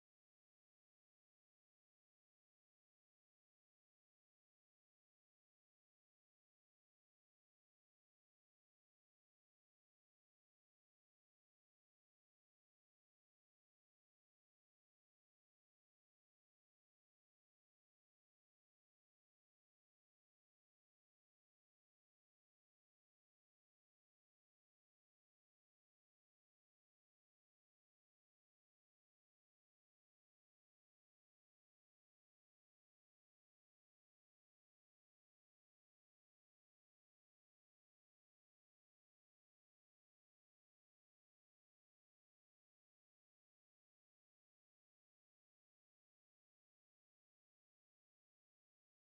tomada 12 V
tomada 12 V ligar aparelhos com polaridade ideal para roletes, barcos, campismo